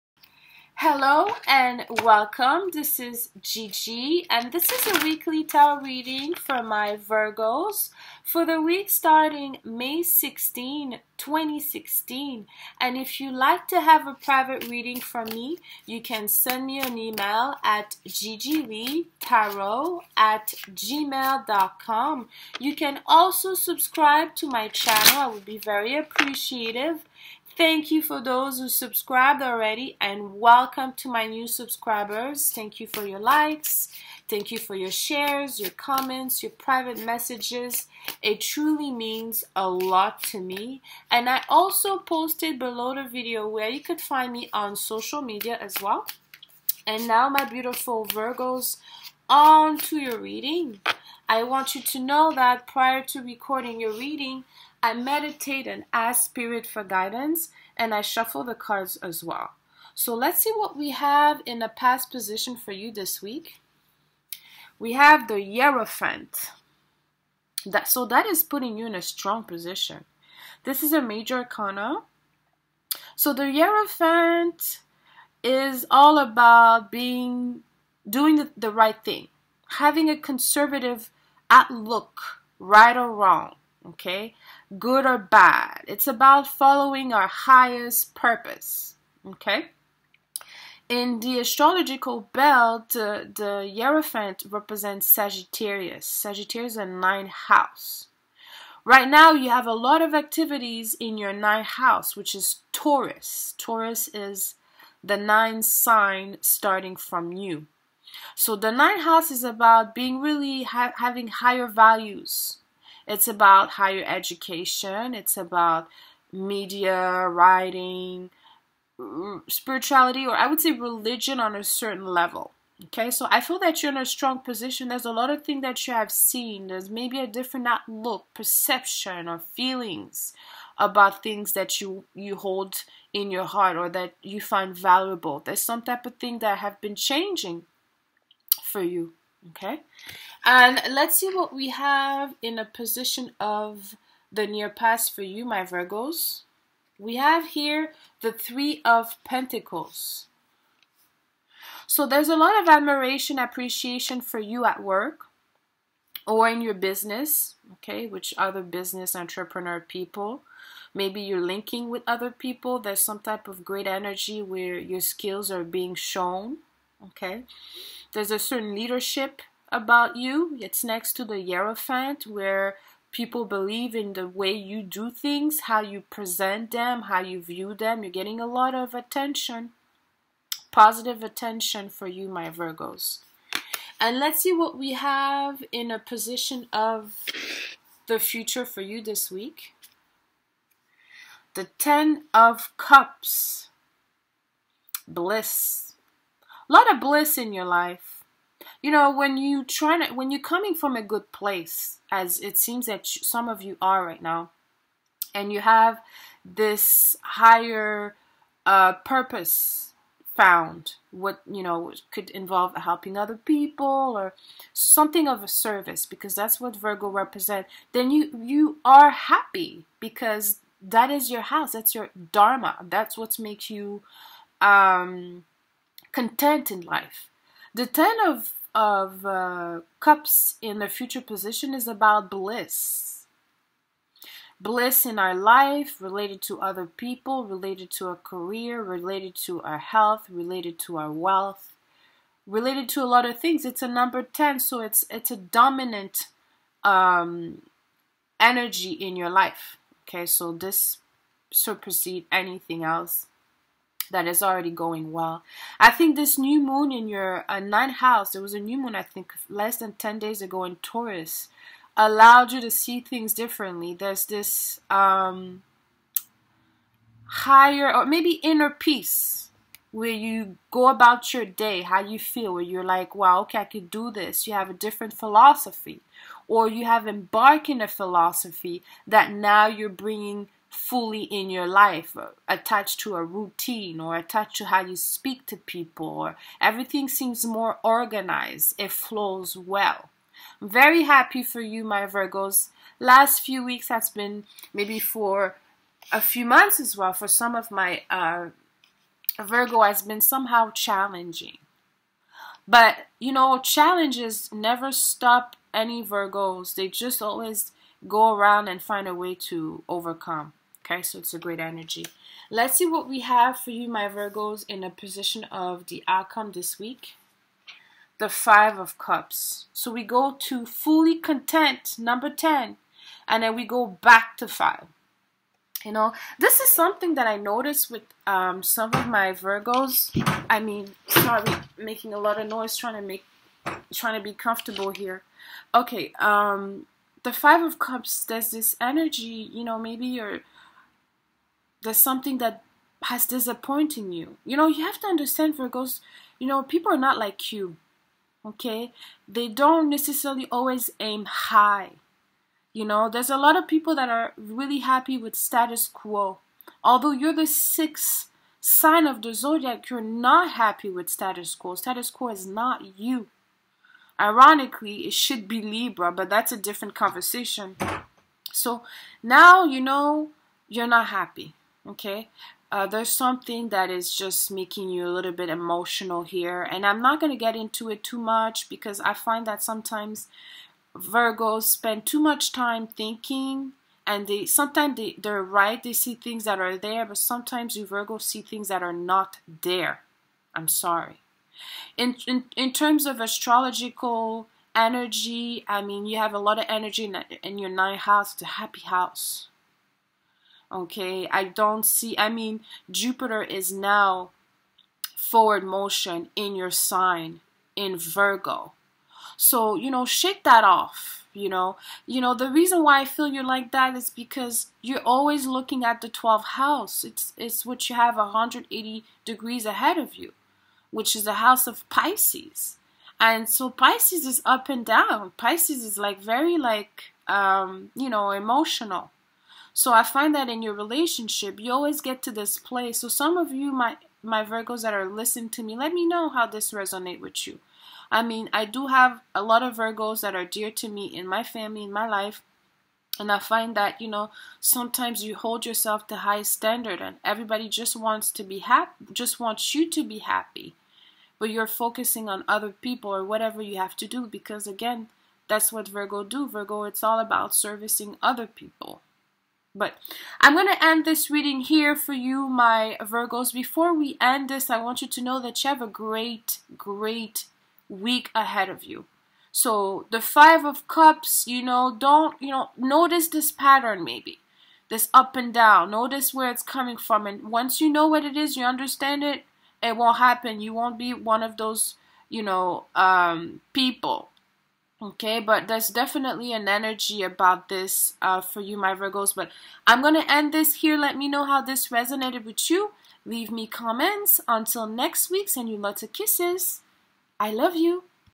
Virgo May 16 to May 22 | Tarot Reading 2016
Gigi Lee, Astrologer, Tarot Card Reader and Spiritual Coach

If You Like This Video, Take a Look at my FB Group  ~GIGI LEE ASTRO TAROT~